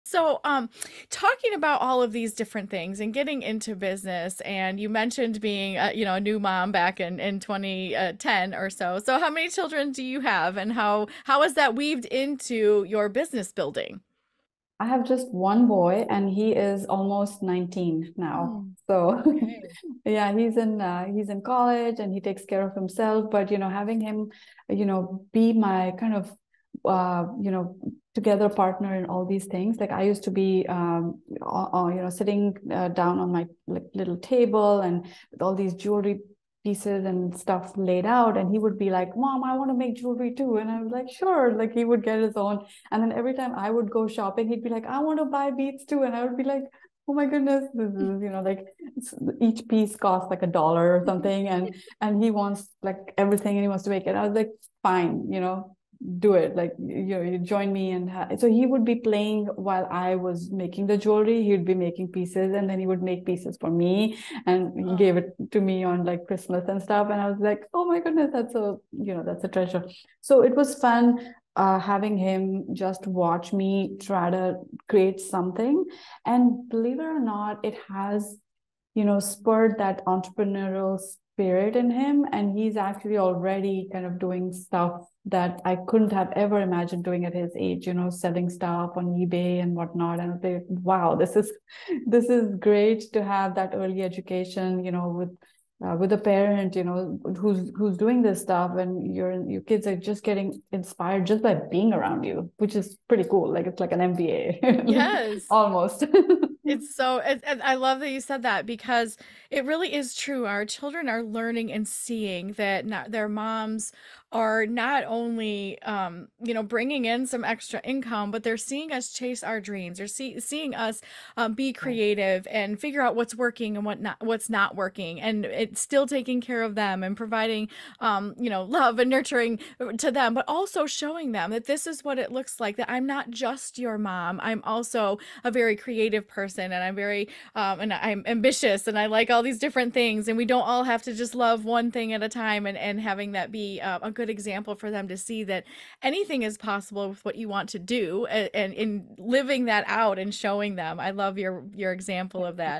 Balancing Entrepreneurship & Parenthood- An Inspirational Journey
Juggling parenthood and entrepreneurship? You're not alone! As a mother to a now college-aged son, Ritu's journey in business building has been tightly woven with raising him. From making jewelry together to nurturing his own entrepreneurial spirit, having him by her side enriched the experience. Her children learn from them, observing our creativity, perseverance, and ambition. They see them not just as caregivers but also as individuals with passions and dreams. More than just generating extra income, we're setting examples and teaching valuable life lessons. This exposure is an early education in entrepreneurship - a practical MBA. By living our dreams, we show our children that anything is possible, painting a vibrant picture of boundless possibilities. As mothers and business owners, we're creating the future, one inspiring example at a time.